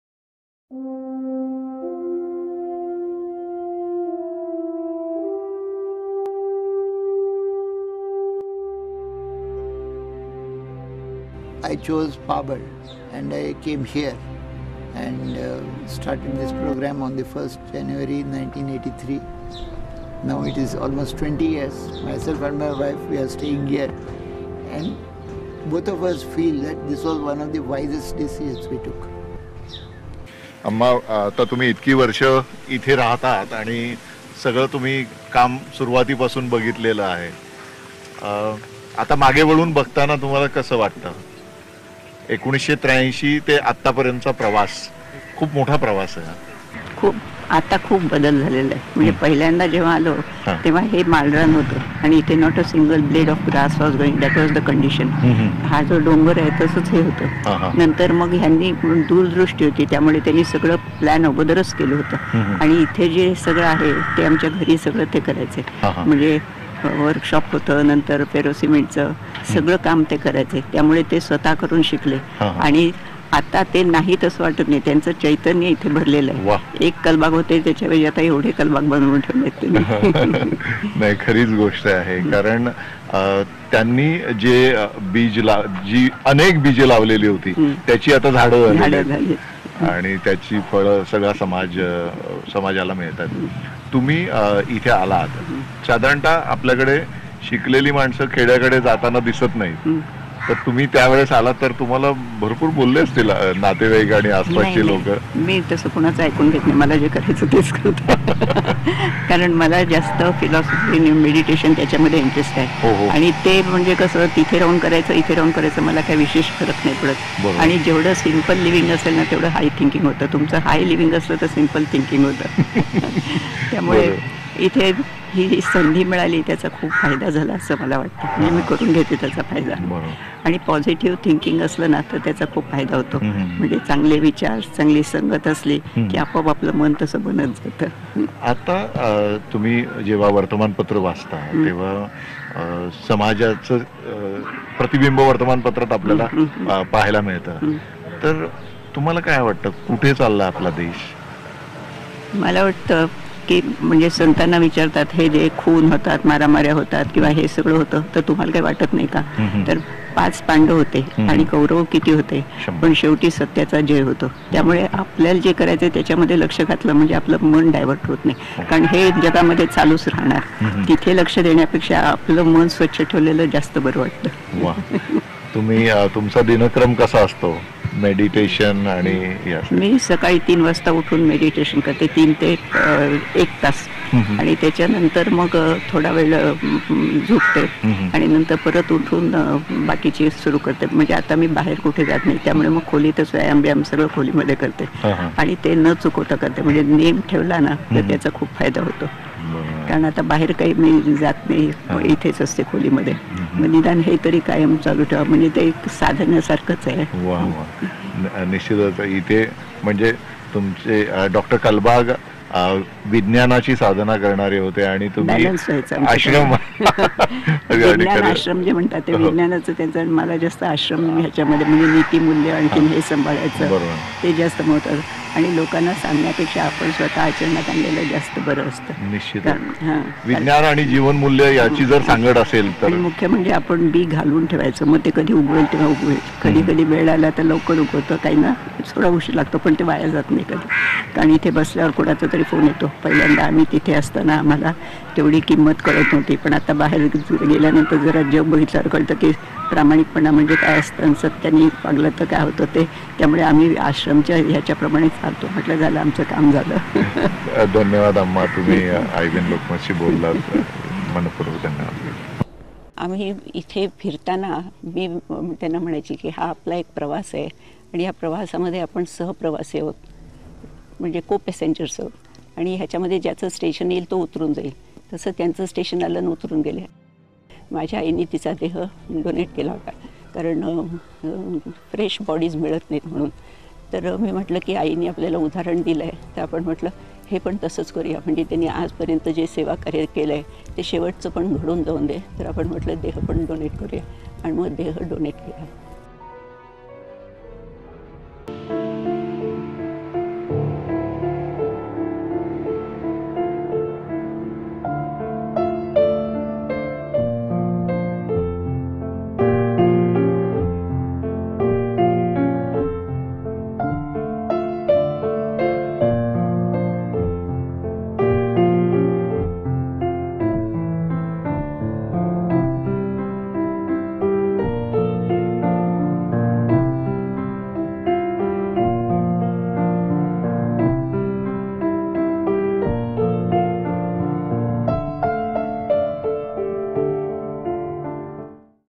Mira Srinath Kalbag Amma from Vigyan ashram Pabal
This video is about our inspiration  Amma (Mira Srinath Kalbag) of Vigyan ashram  .